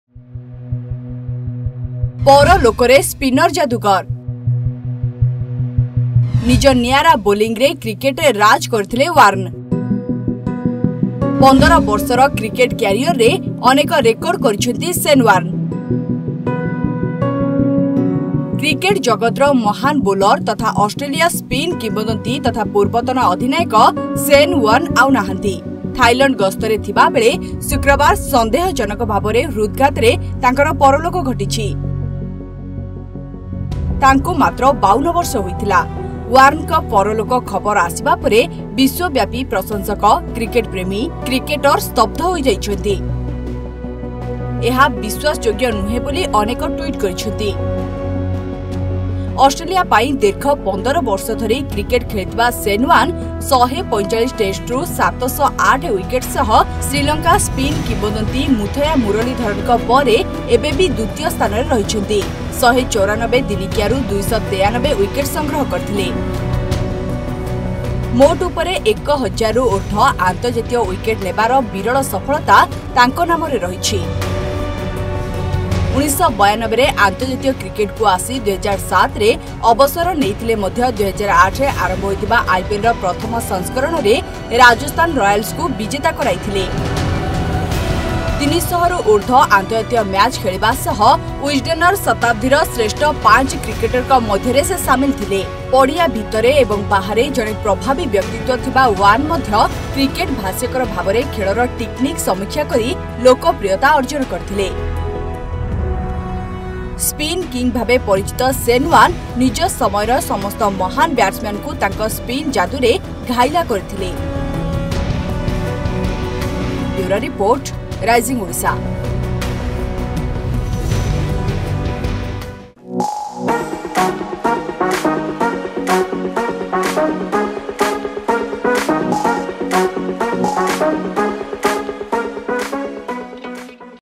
Shocking! Australian Cricket Legend Shane Warne Passes Away At 52 | ଅଲବିଦା ସ୍ପିନ୍ କିଙ୍ଗ
Australian Cricket Legend Shane Warne Passes Away At 52 Suffering from Cardiac Arrest.